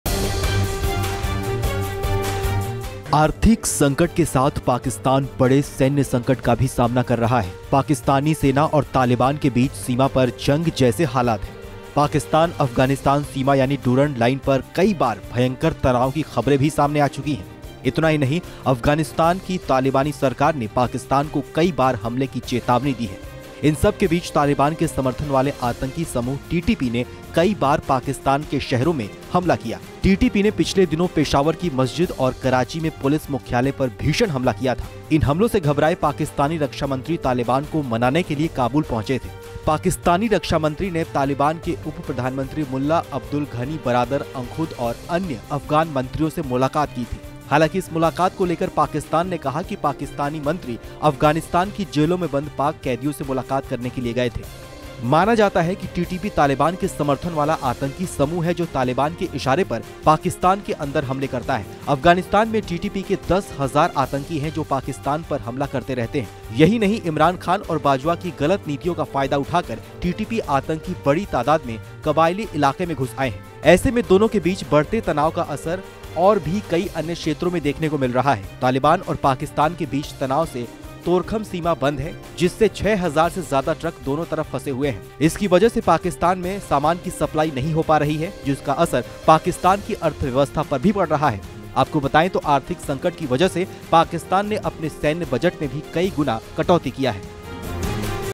बड़ी मुश्किल में फंस गई पाकिस्तानी सेना, अब बचना नामुमकिन ! | Pakistan Army | Taliban | PoK | Kashmir
बीते कुछ महीनों में पाकिस्तान में कई बड़े आतंकी हमले हुए..पाकिस्तान में हुए इन आतंकी हमलों में सैकड़ों लोग मारे गए... पाकिस्तान में इन हमलों की जिम्मेदारी तालिबान के समर्थन वाले आतंकी समूह TTP ने ली, TTP वही आतंकी समूह है जिसने अब से कुछ महीने पहले ही बयान जारी कर कहा था कि वो पाकिस्तान को बर्बाद कर देगा।

In the last few months, there were many major terrorist attacks in Pakistan..Hundreds of people were killed in these terrorist attacks in Pakistan..The responsibility of these attacks in Pakistan was taken by Taliban supported terrorist group TTP, TTP is the same terrorist group which has now A few months before, he had issued a statement saying that he would destroy Pakistan.